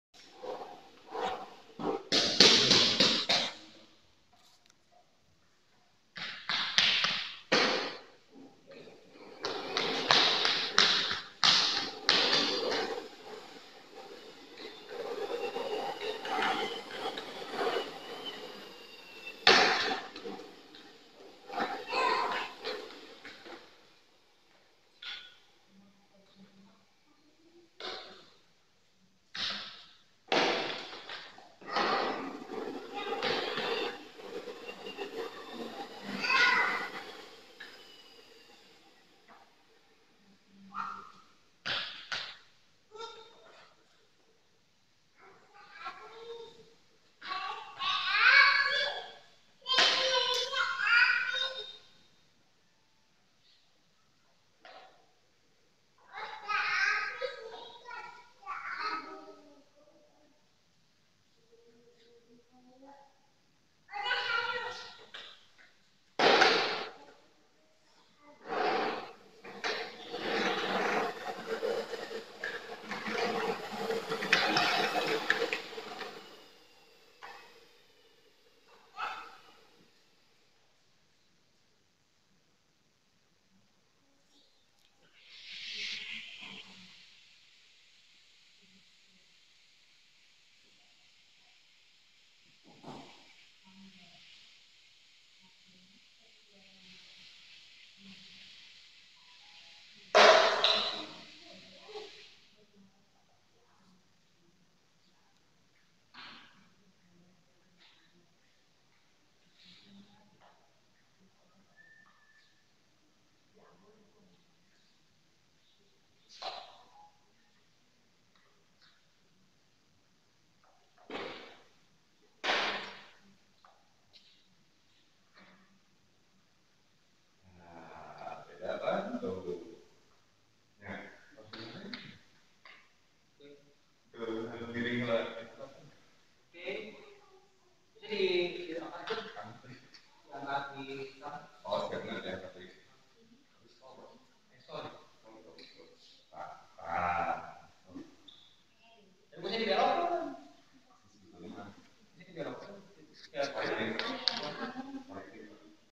Pesawat Tenaga Karet Indoor Limited Pennyplane
Pesawat Tenaga Karet Indoor Limited Pennyplane dengan putaran 400x
berat 3,6gr